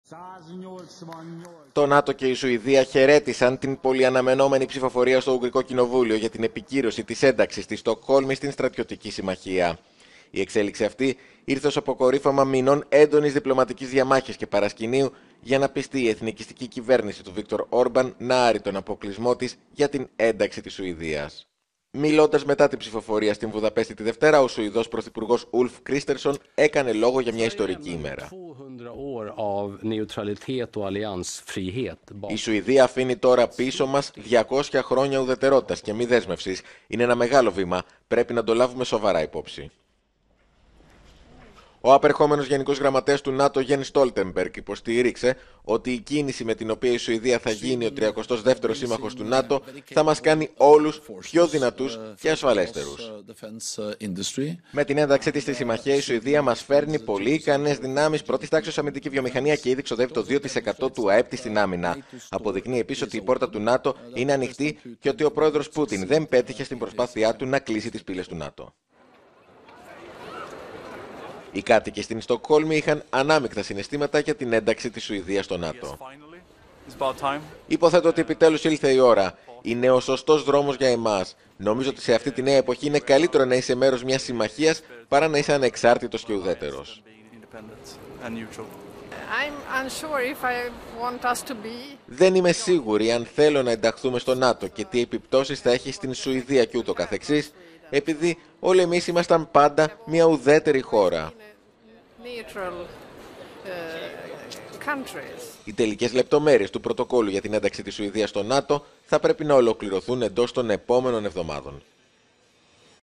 Η επόμενη μέρα της ουγγρικής επικύρωσης της ένταξης της Σουηδίας στο ΝΑΤΟ
Ανάμεικτα συναισθήματα στους πολίτες για την εγκατάλειψη της ουδετερότητας